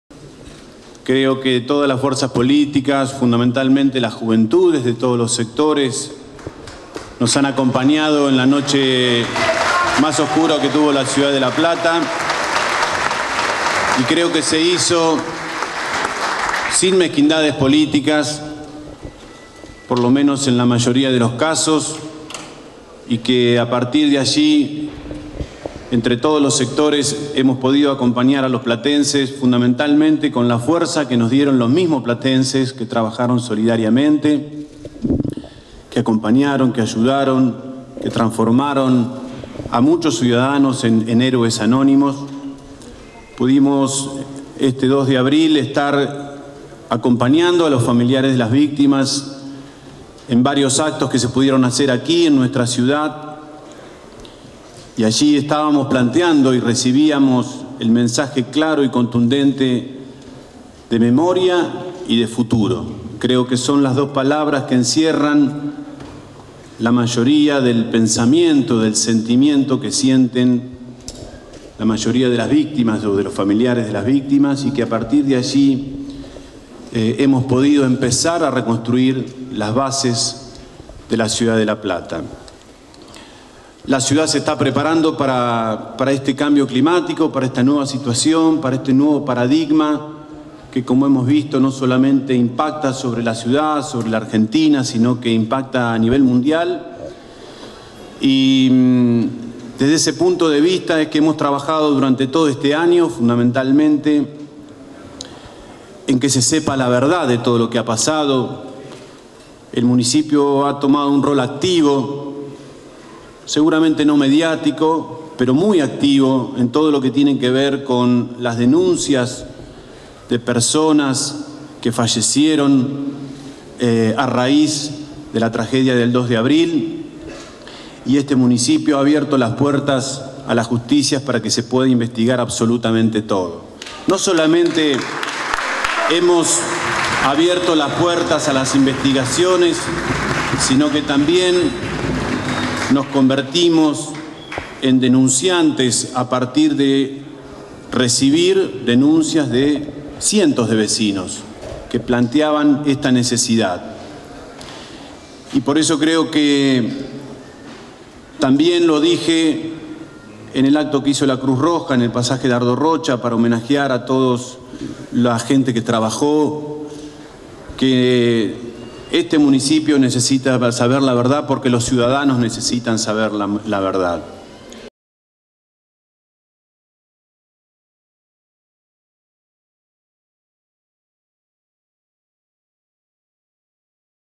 BALANCE DE GESTION MUNICIPAL
EL INTENDENTE DE LA CIUDAD, PABLO BRUERA DEJO INAUGURADA LAS SESIONES ORDINARIAS EN EL CONCEJO DELIBERANTE CON UN DISCURSO BASADO EN LAS OBRAS QUE SE ESTAN REALIZANDO Y CONTINUARAN HACIENDO A LO LARGO DEL AÑO. ADEMAS DEDICO UN MENSAJE A LOS FAMILIARES DE LAS VICTIMAS DE LA TRAGEDIA DEL 2 DE ABRIL DE 2013.